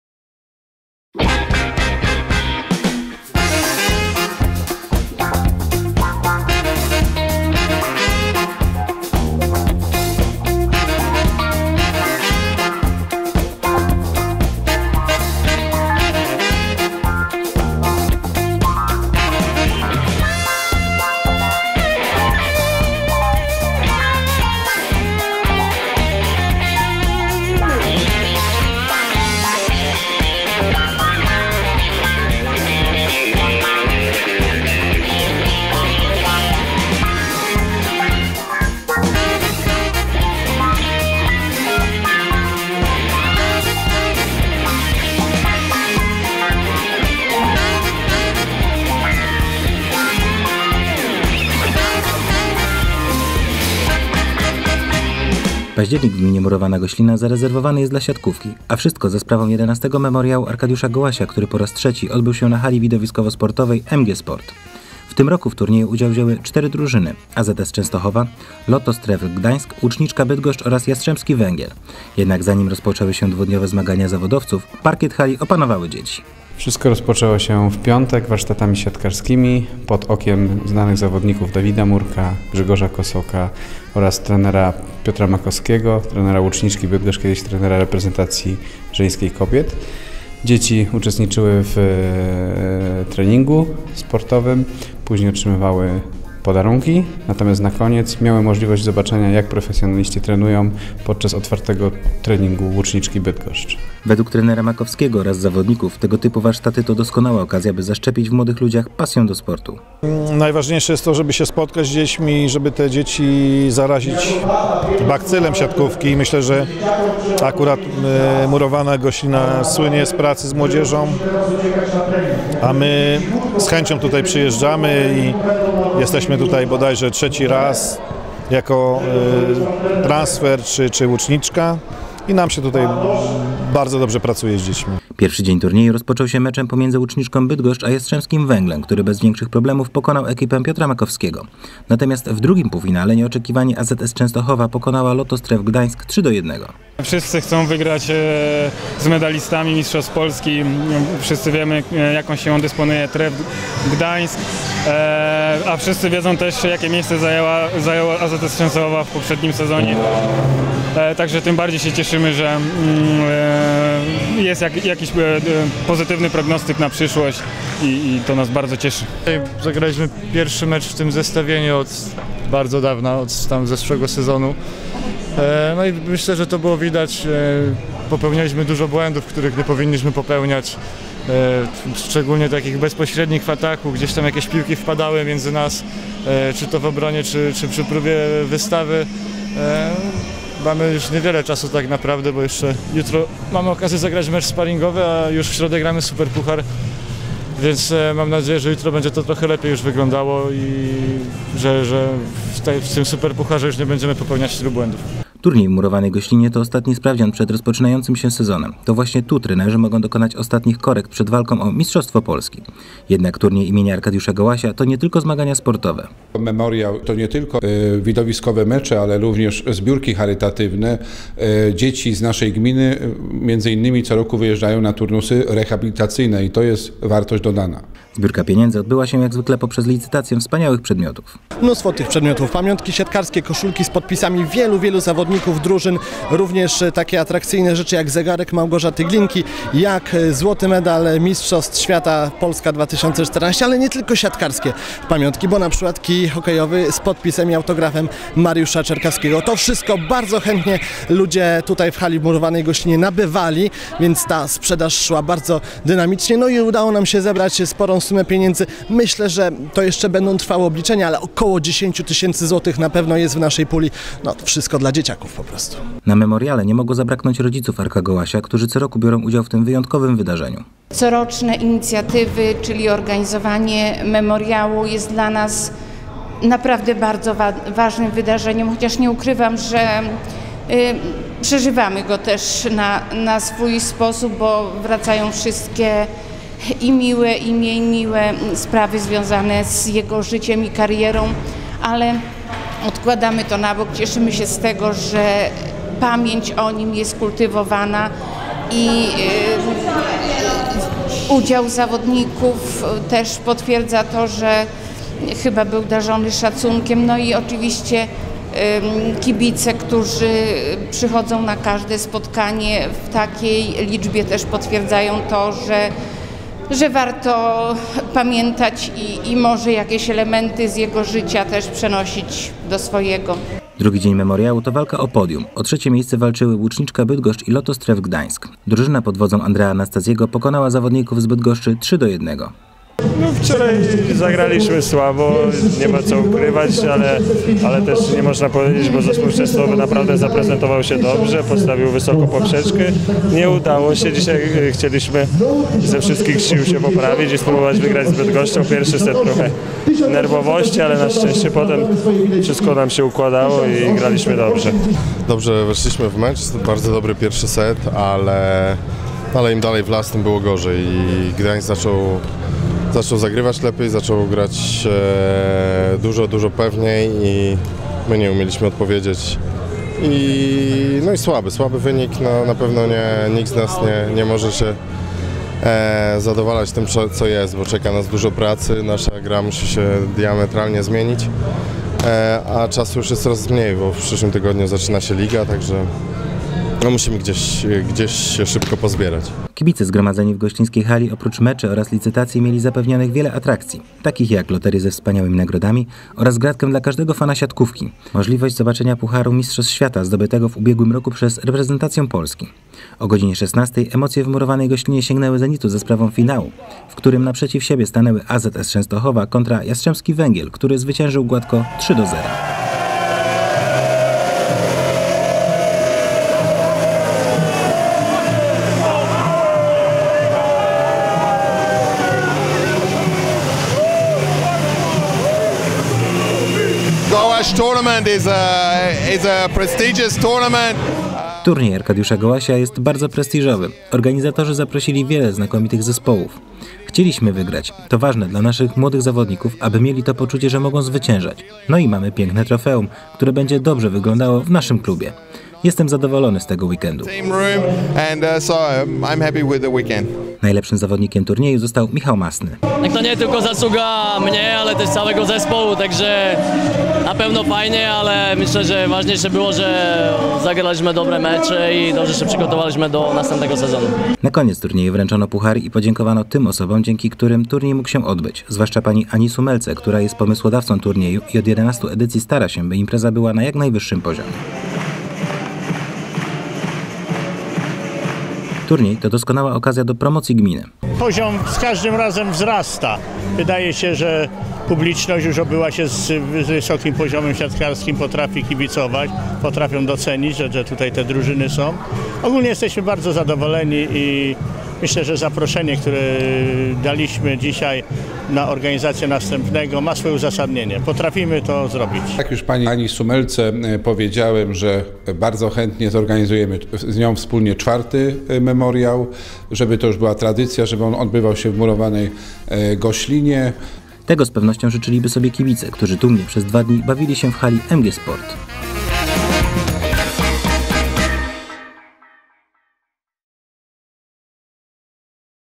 XI Memoriał im Arkadiusza Gołasia w Murowanej Goślinie
Już po raz trzeci w Murowanej Goślinie odbył się memoriał Arkadiusza Gołasia. Jest to święto siatkówki i niebywała okazja by przeżyć emocje na najwyższym sportowym poziomie. Tegoroczny memoriał odbył się w dniach 24-25 października.